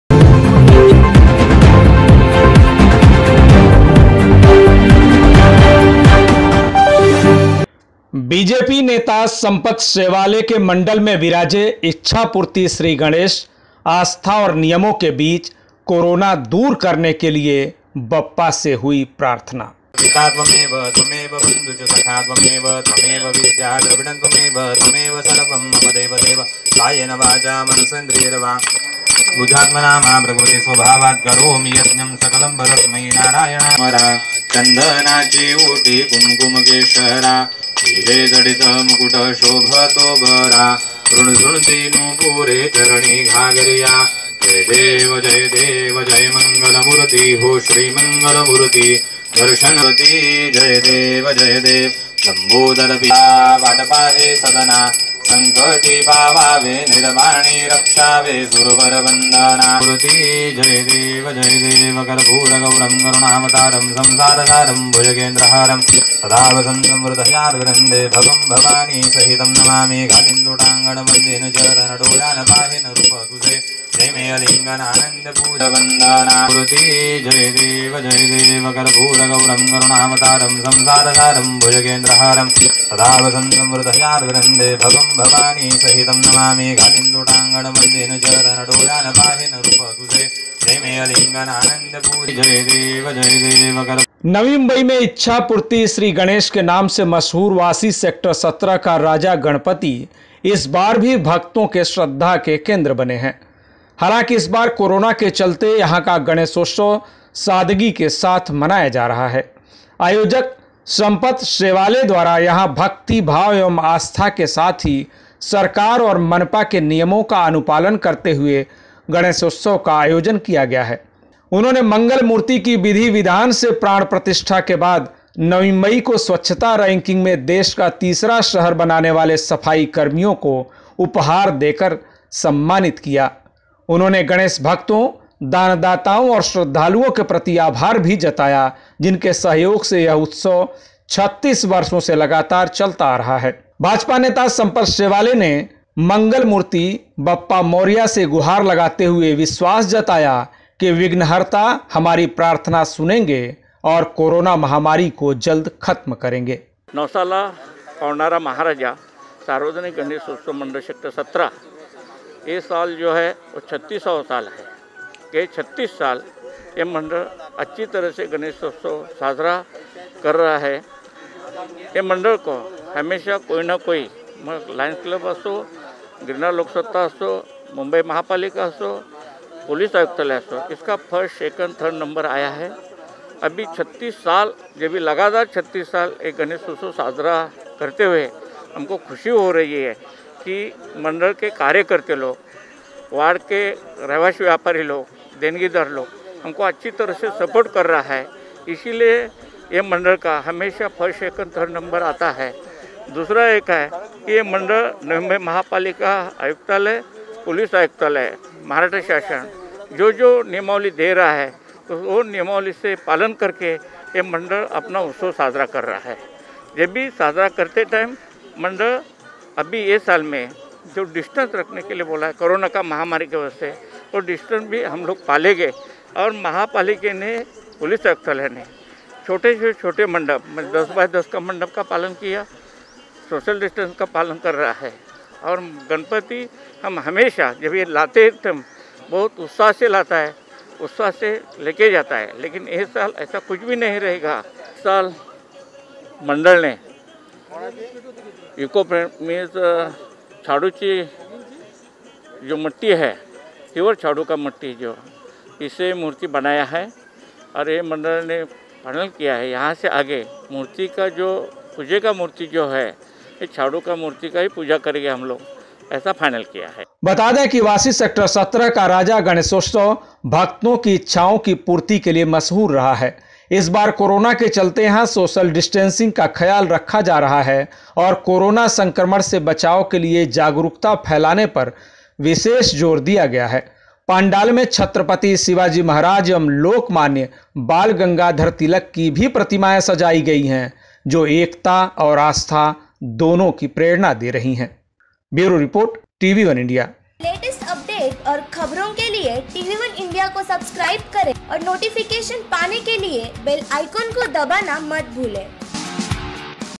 BJP नेता संपत शेवाले के मंडल में विराजे इच्छापूर्ति श्रीगणेश, सफाई कर्मियों का उपहार देकर सत्कार
नवी मुंबई में इच्छापूर्ति गणेश के नाम से मशहूर वाशी सेक्टर 17 का राजा गणपति इस बार भी भक्तों के श्रद्धा के केन्द्र बने हैं. हालांकि इस बार कोरोना के चलते यहां का गणेशोत्सव सादगी के साथ मनाया जा रहा है. आयोजक संपत शेवाले द्वारा यहां भक्ति भाव एवं आस्था के साथ ही सरकार और मनपा के नियमों का अनुपालन करते हुए गणेशोत्सव का आयोजन किया गया है.